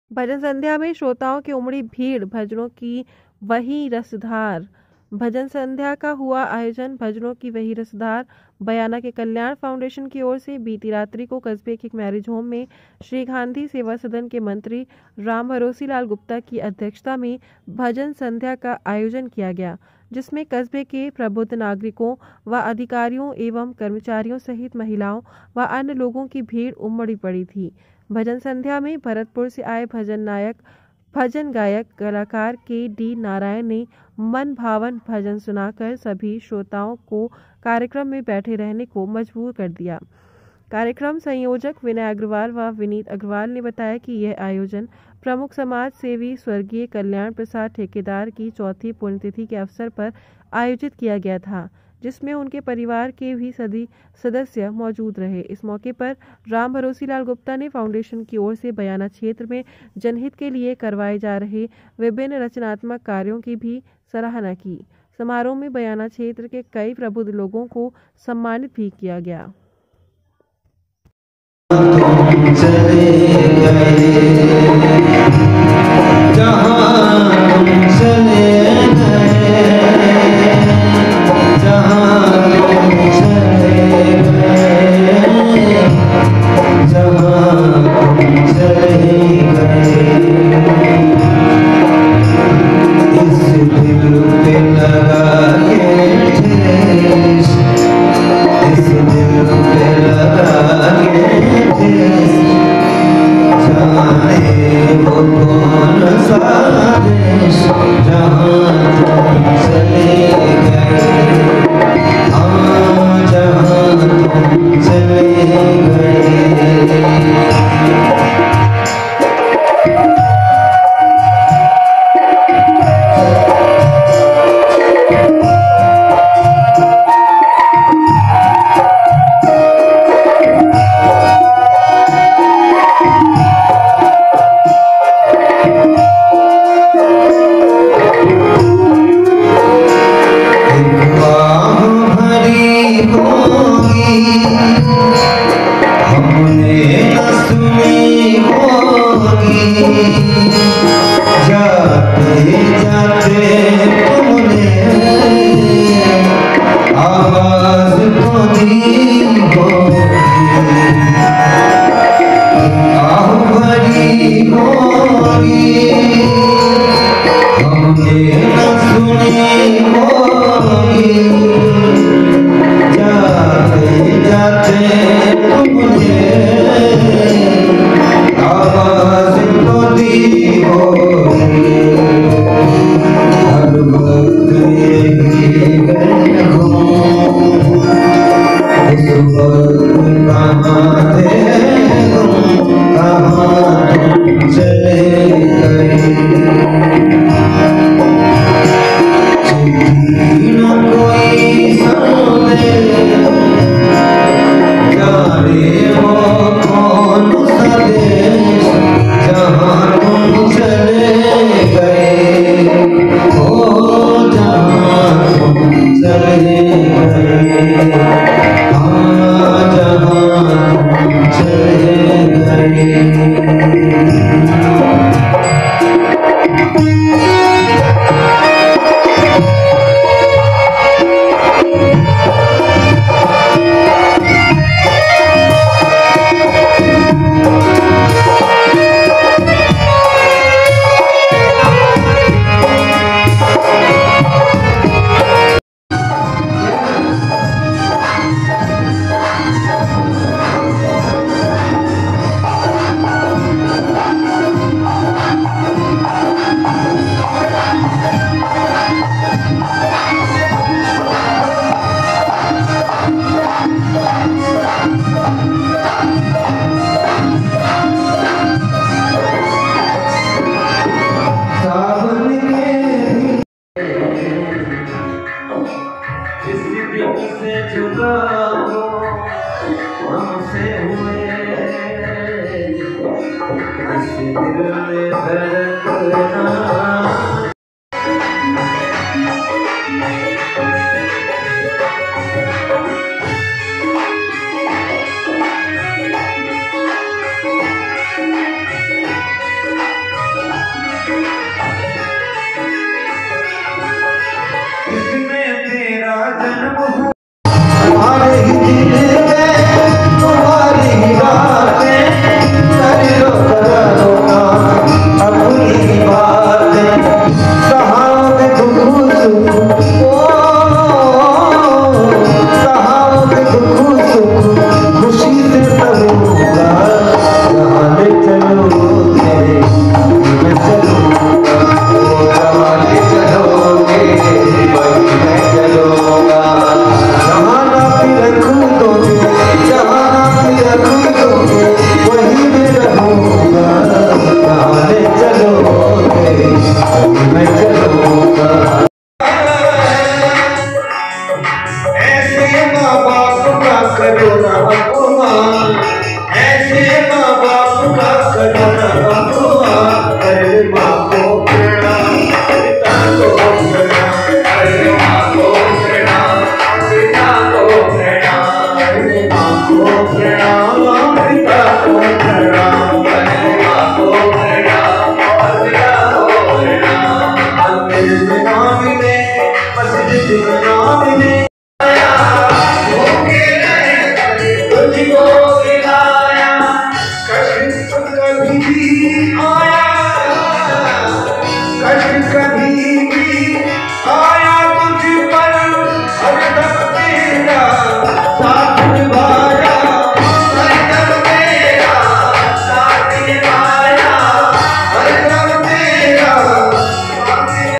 संध्या में श्रोताओं की उमड़ी भीड# भजनों की वहीं रसधार#gnewsindia#राजस्थान#
बयाना 21 नवम्बर। भजन संध्या का हुआ आयोजन, भजनों की वही रसधार,बयाना के कल्याण फाउंडेशन की ओर से बीती रात्री को कस्बे के एक मैरिज होम में श्रीगांधी सेवा सदन के मंत्री रामभरोसी लाल गुप्ता की अध्यक्षता में भजन संध्या का आयोजन किया गया जिसमें कस्बे के प्रबुद्ध नागरिकों व अधिकारीयों एवं कर्मचारीयों सहित महिलाओं व अन्य लोगों की भीड उमड पडी थी। भजन संध्या में भरतपुर से आए भजन गायक कलाकार के डी नारायण ने मनभावन भजन सुनाकर सभी श्रोताओं को कार्यक्रम में बैठे रहने को मजबूर रखा। कार्यक्रम संयोजक विनय अग्रवाल व विनीत अग्रवाल ने बताया कि यह आयोजन प्रमुख समाज सेवी स्व.कल्याण प्रसाद ठेकेदार की चौथी पुण्यतिथी के अवसर पर आयोजित किया गया था। जिसमें उनके परिवार के भी सभी सदस्य मौजूद रहे। इस मौके पर रामभरोसीलाल गुप्ता ने फाउंडेशन की ओर से बयाना क्षेत्र में जनहित के लिए करवाए जा रहे विभिन्न रचनात्मक कार्यों की भी सराहना की। समारोह में बयाना क्षेत्र के कई प्रबुद्ध लोगों को सम्मानित भी किया गया।